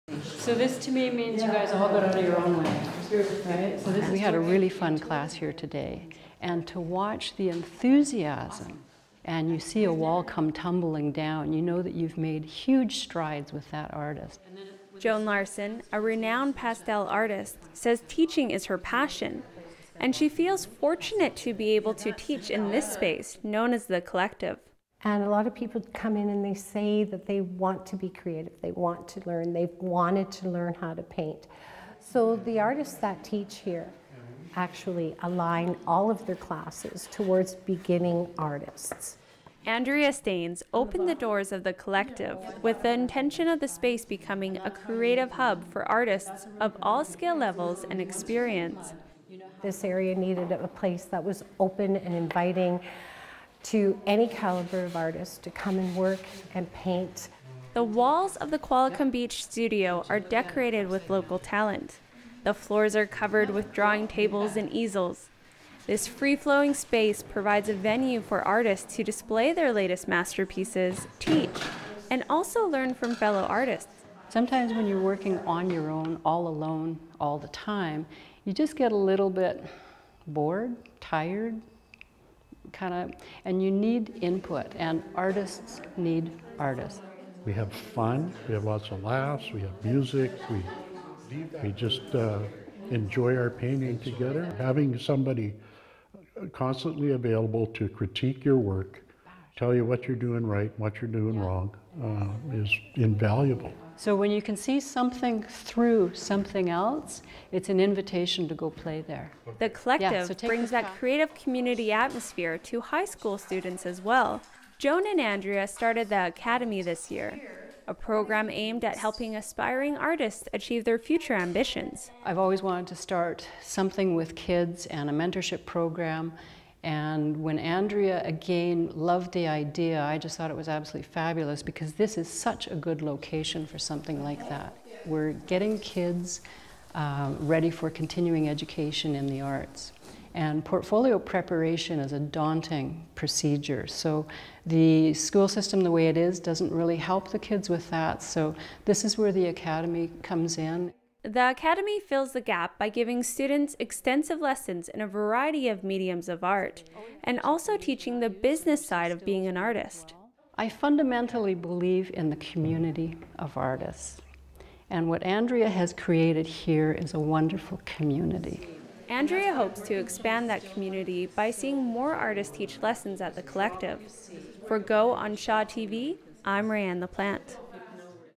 The Collective Art Co-operative - Shaw TV Nanaimo
The Collective is a new artist cooperative in Qualicum Beach teaching emerging and enthusiastic artists how to be creative.